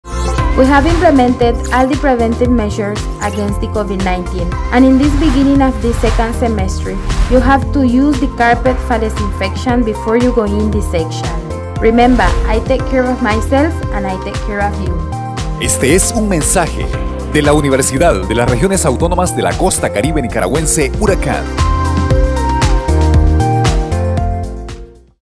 KRIOL I Hou tu iuz di caarpets: Ai tek kier af maiself an Ai tek kier af yuu
Wi hav implemented aal di preventiv meshurs  ageens di covid 19 an  in dis biginin af dis sekant semester yu hav tu iuz di caarpets fa desinfekshan bifuor yu go iin to di sekshan.

Rimemba: Ai tek kier af maiself an Ai tek kier af yuu